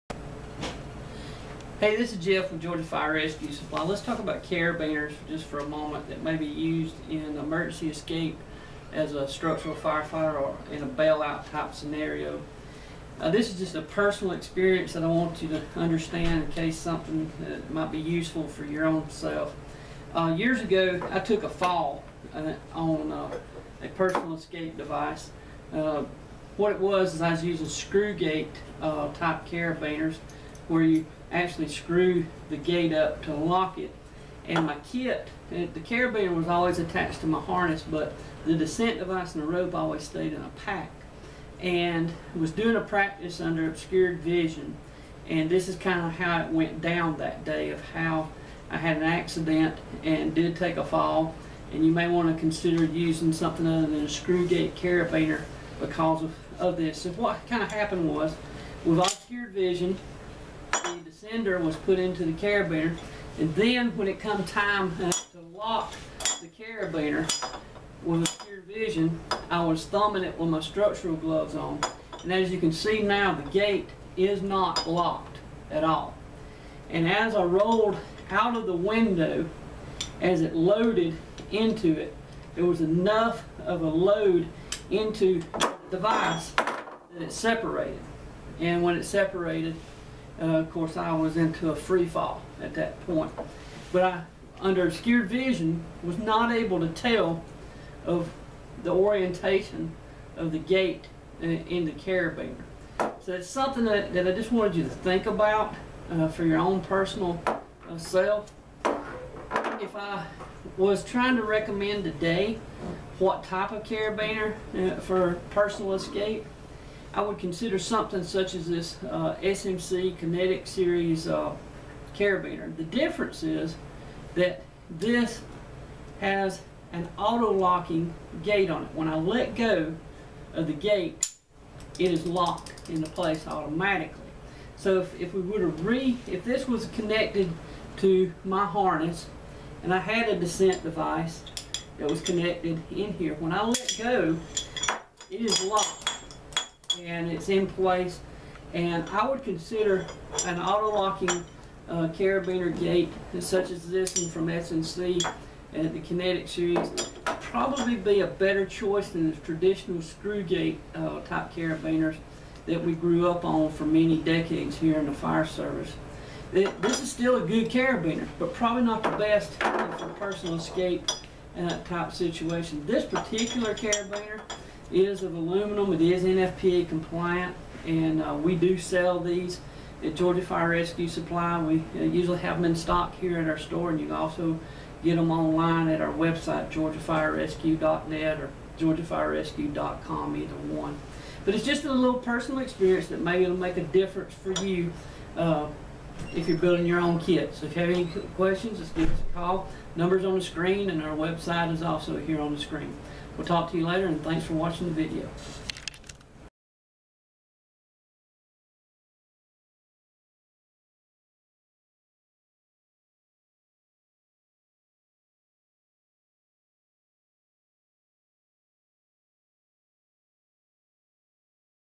Carabiner selection for emergency escape (bailout)
Here I review a fall I took while practicing with a bailout pack under obscured vision with a human error in the use of a carabiner with a screw gate. We suggest using autolock carabiners for the use of firefighter emergency escape (bailout). The autolock carabiner in the video is the SMC Kinetic series carabiner.